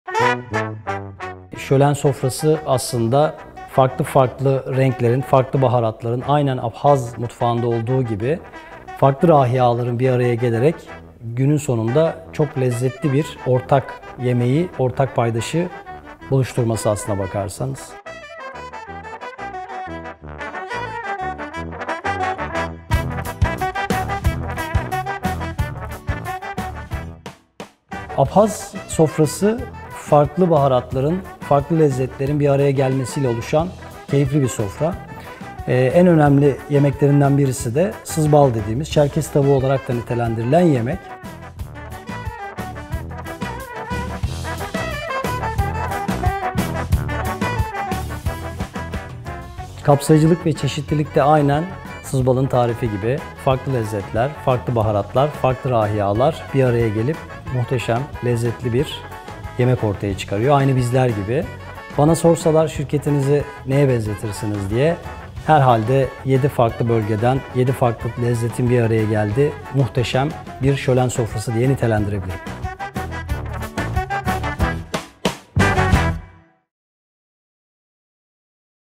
Mey|Diageo Etkinlik & On Trade Müdürü Halil Önder Arıt Şölen Sofrası projesini anlatıyor
Dede ve babaannesiyle paylaştığı her sofra deneyiminin bayram sabahları kadar kıymetli olduğunu dile getiren Mey|Diageo Etkinlik & On Trade Müdürü Halil Önder Arıt bu sofraların sadece yemek amaçlı değil, deneyimlerin aktarıldığı seanslara da hizmet ettiğini söylüyor. "Kapsayıcılık da aynı lezzetli tarifler gibi. Yemeği özel ve lezzetli yapan şey, farklı malzemelerin ve baharatların bir araya gelmesi ve lezzetli bir bütünü oluşturması. Aynı bizler gibi...’’ diyor ve annesinden aldığı sızbal tarifini anlatıyor.

Kendini gelenekleri, öyküleri, kimlikleri kısacası işe giderken beraberlerinde götürdükleri tüm farklılıkları kucaklayan bir şölen sofrasına benzetiyor Mey|Diageo ve Şölen Sofrası projesi için buradan aldığı ilhamla yola çıkıyor. Çeşitlilik ve kapsayıcılığı kurum kültürünün baş sırasında kodlayan Mey|Diageo çalışanlarının geleneksel yemek tariflerinin yer aldığı Şölen Sofrası kitabında Bosna’dan Antakya’ya, Girit’ten Kafkaslar’a kadar geniş bir coğrafyadan gelen 250’den fazla reçete var. Aslında bir yemek kitabından daha çok çalışanlarının kendi kökenlerini ya da ait hissettikleri kültürleri kutladıkları etnik ve coğrafi çeşitlilik derlemesi de diyebiliriz bu kitap için. Geçmişten alınan mirası gelecek nesillere aktarma misyonuyla hareket eden Şölen Sofrası’nda öne çıkan lezzetlere, arkalarında yatan hikayelerine ve birbirinden zengin sofralara dadanıyoruz.

Video Proje: Dadanizm / İris Işık & Seden Mestan & Melisa Su Akar
Yönetmen: Kaan Temizkan
Fotoğraf: Büşra Yeşilay